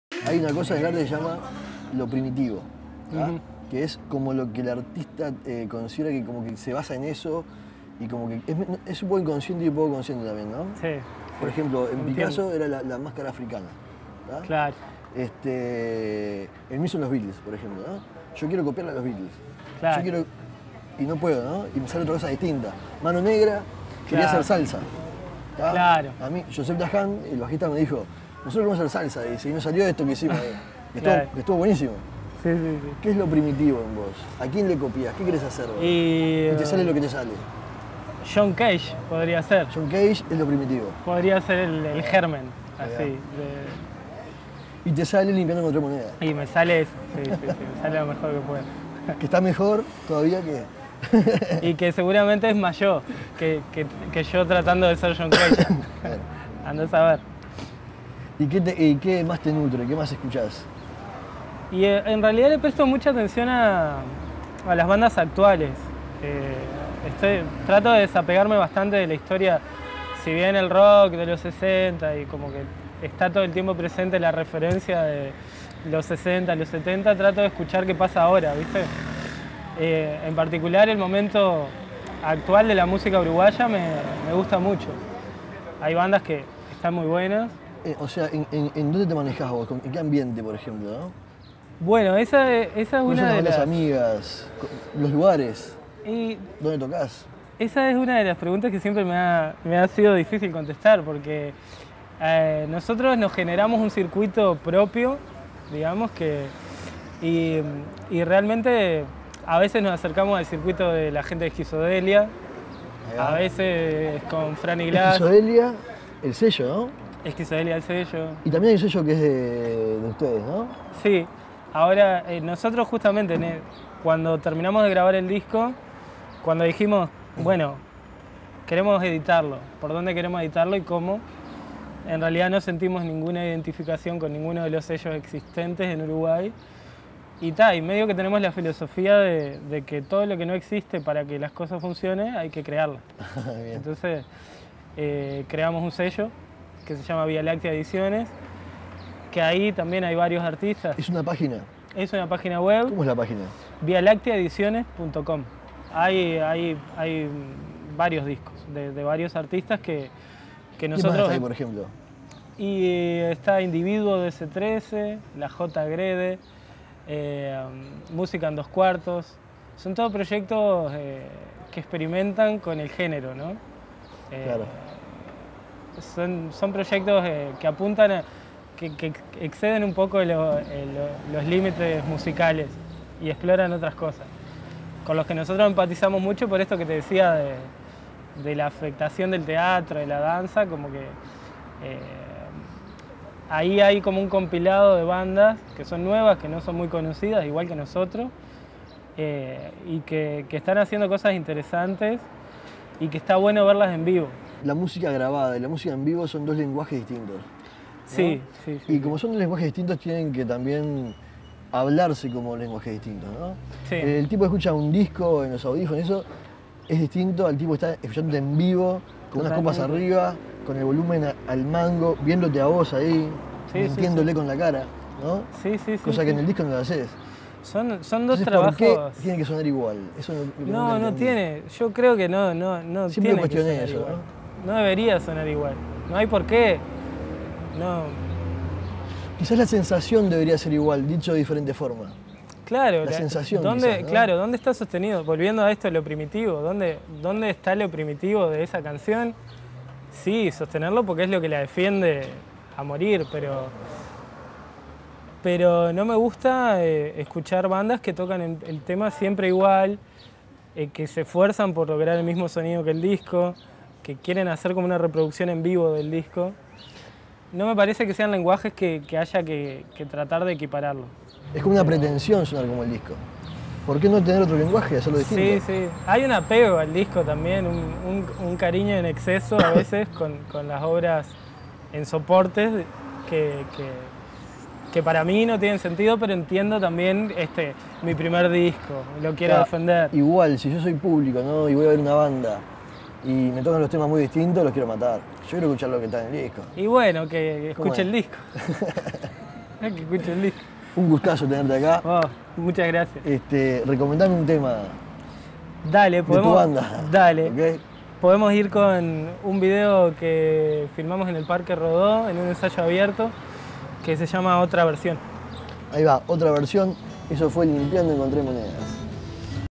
Salvador García (Limpiando Encontré Monedas), Parte 2
Conversa sobre folk, indie y canción popular con Salvador García de los LIMPIANDO ENCONTRÉ MONEDAS y Max Capote en La Ronda. Blister, 2013.